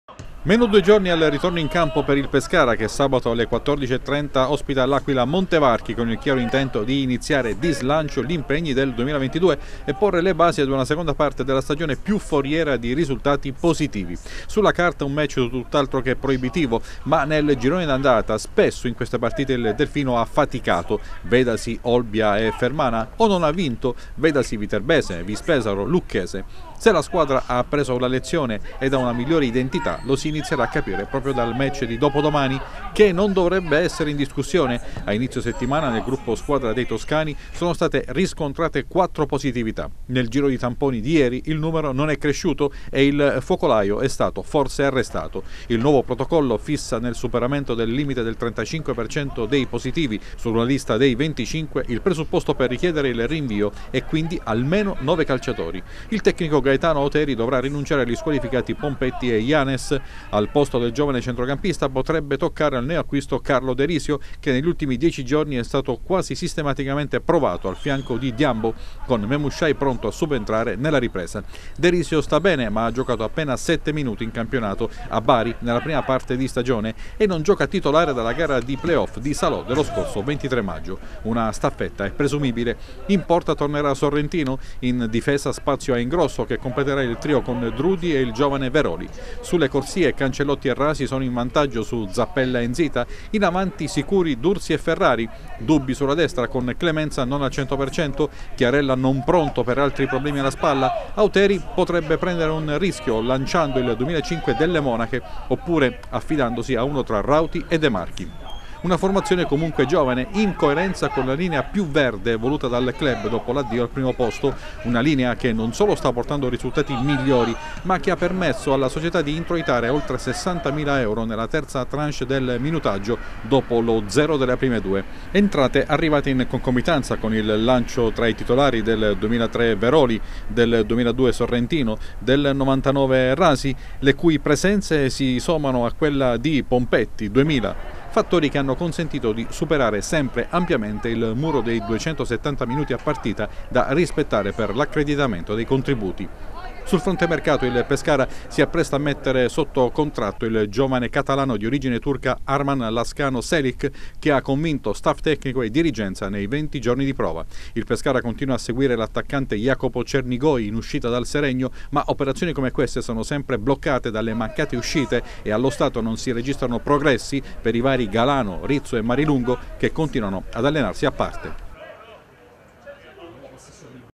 Pescara: la linea verde spinge risultati e minutaggio
Dopodomani alle 14:30 il Pescara ospiterà il Montevarchi all’Adriatico. Assenti Pompetti e Illanes, il tecnico Auteri potrebbe lanciare dal primo minuto il neo centrocampista De Risio. Sarà comunque un Pescara giovane come lo è stato negli ultimi due mesi di stagione, con benefici per i risultati e anche per i contributi del minutaggio